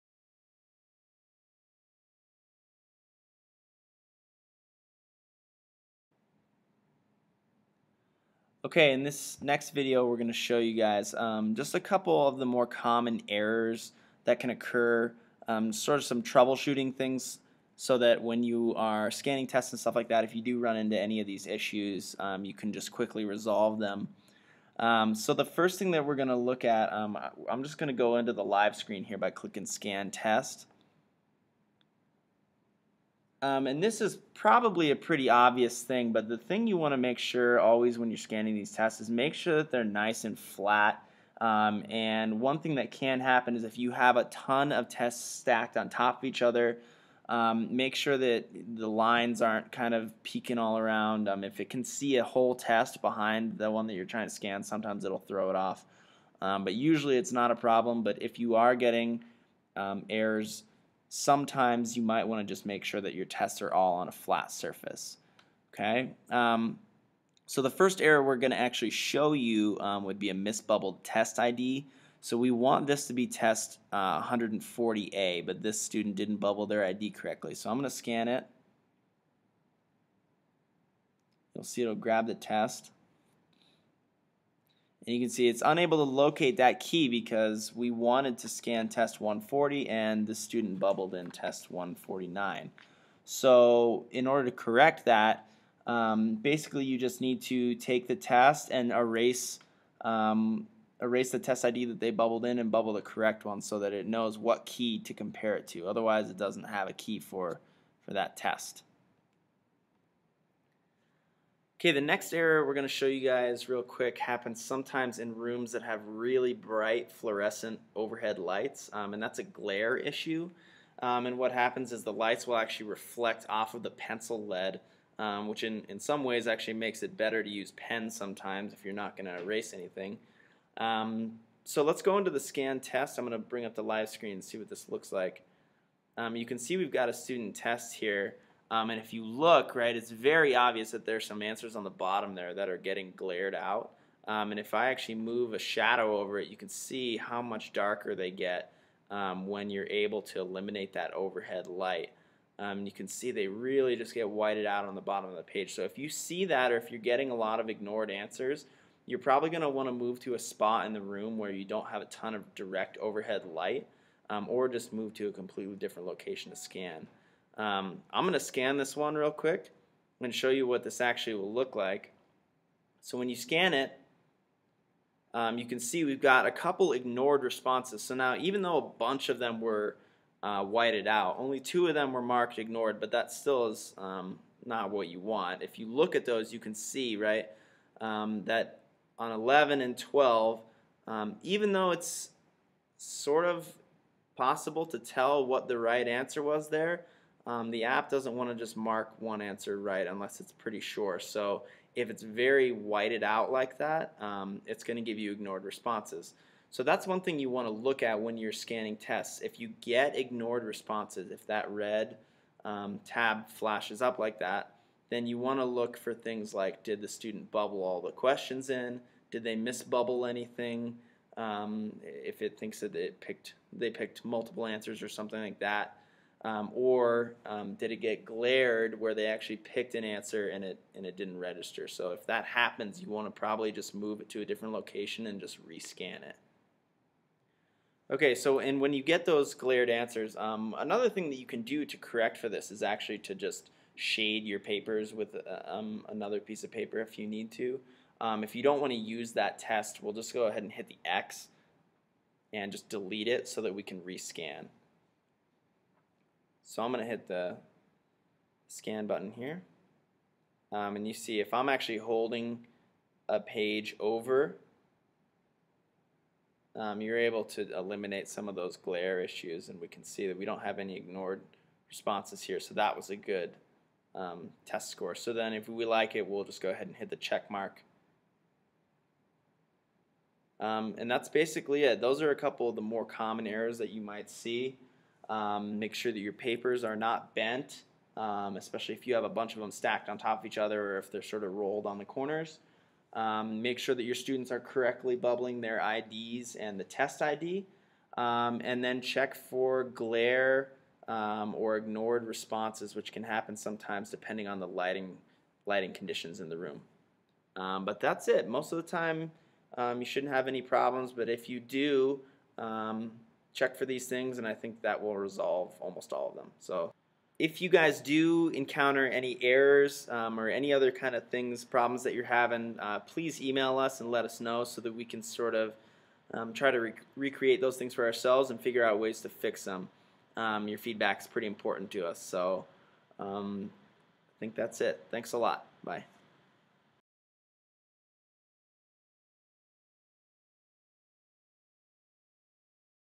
WISE App Setup Part 4: Troubleshooting Common Errors
This video examines possible errors you might occur and ways to resolve them.  These include:
Bent Page
Mis-bubbled ID or Test ID
Glare and Ignored Responses